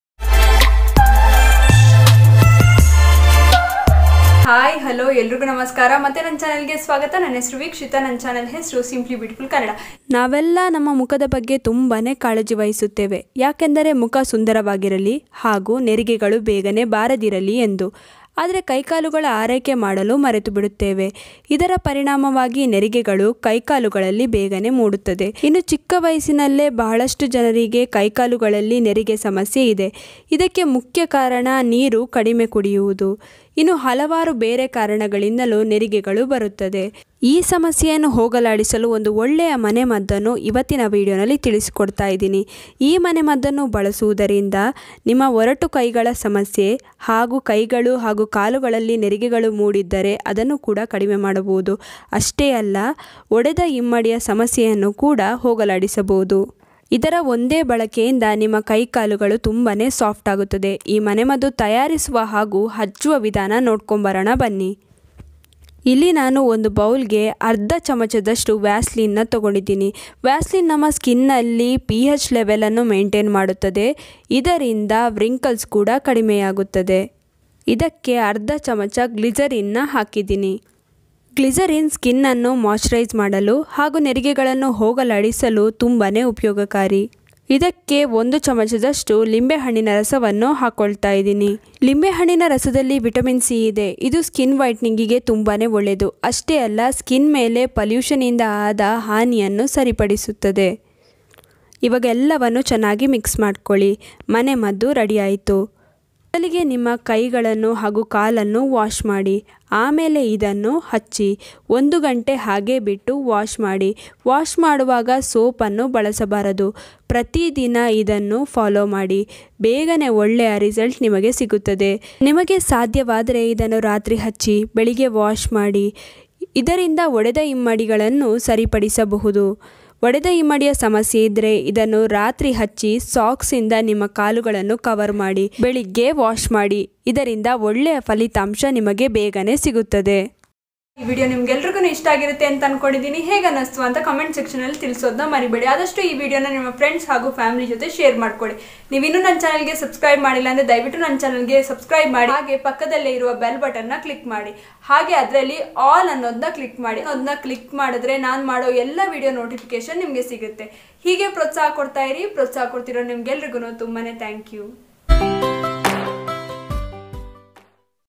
ಕೈ ಕಾಲುಗಳಲ್ಲಿ ನೆರಿಗೆ ಇದ್ದರೆ ಇದನ್ನು ಬಳಸಿ | How to Remove Wrinkles in Hands and Legs | Beauty Tips
Hi Everyone,